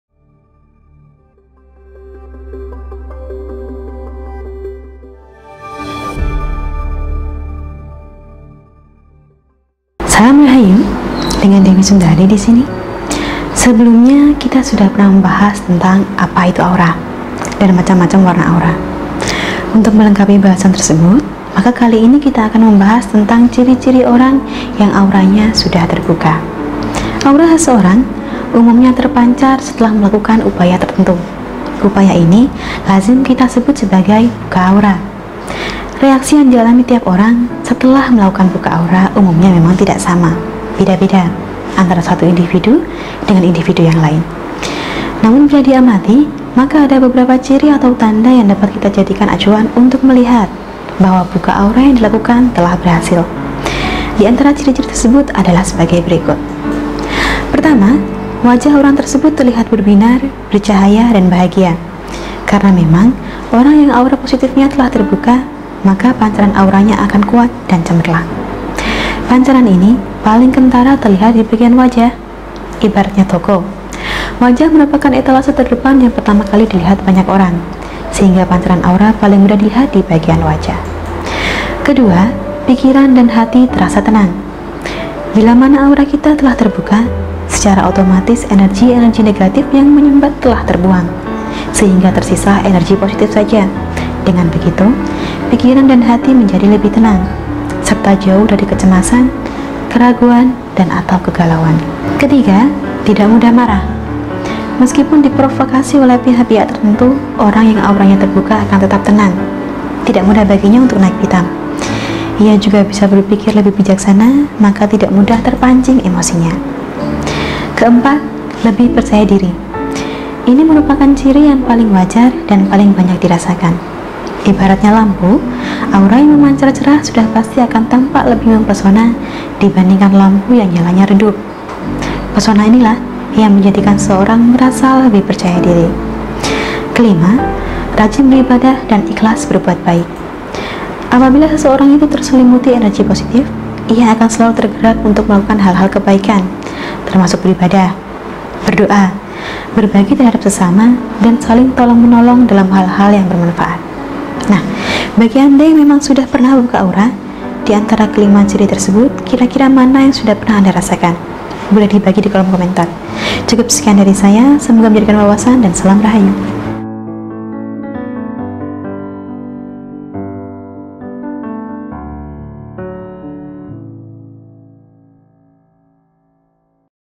Begini Ciri ciri Orang Yang Auranya Sudah Terbuka - Dewi Sundari
Bahasan saya kali ini adalah tentang apa saja ciri ciri orang yang auranya sudah terbuka. Simak pembahasannya berikut.